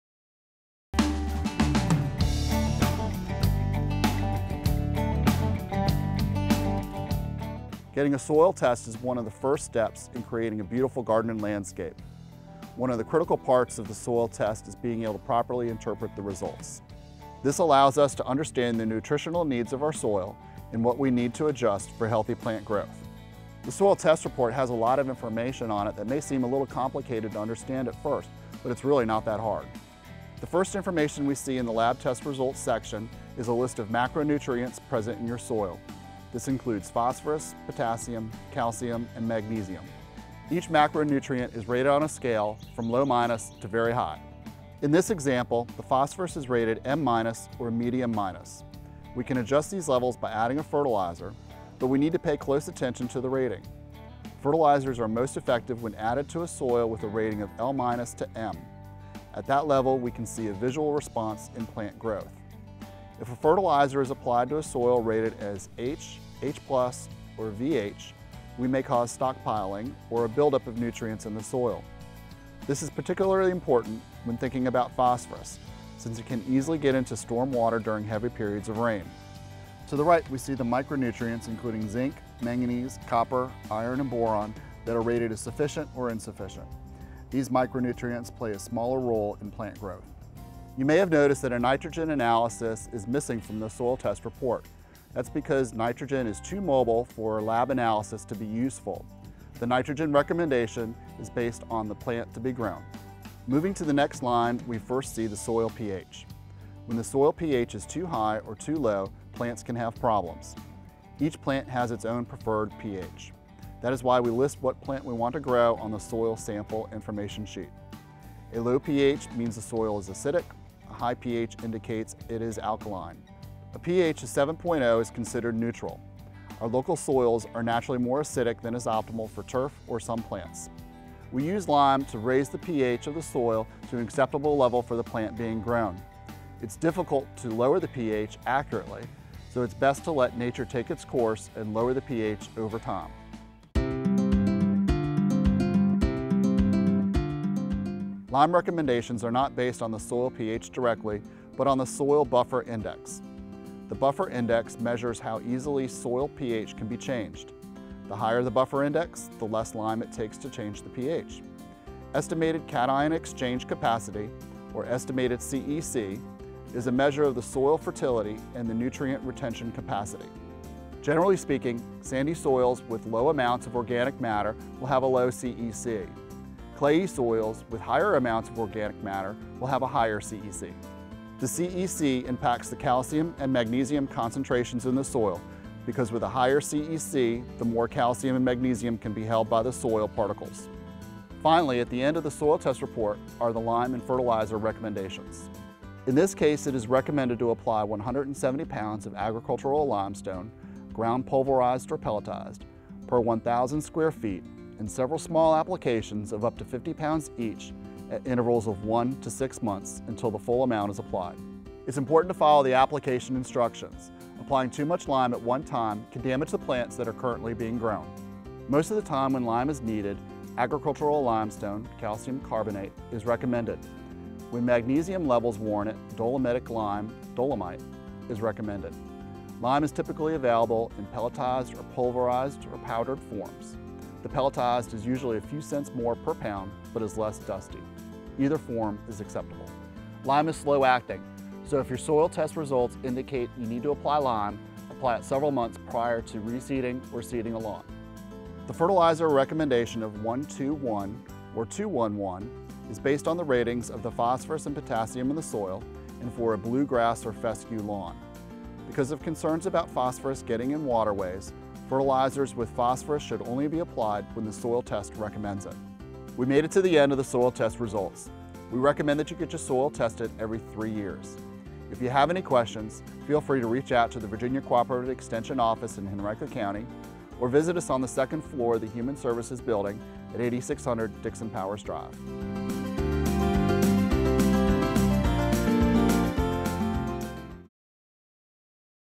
Interpreting Your Soil Test
A basic soil test can reveal the nutritional needs of your property and provide everything you'll need to know to create a healthy lawn and landscape. From A to Z (alkaline to zinc), the Henrico office of Virginia Cooperative Extension explains how to make sense of your soil test results.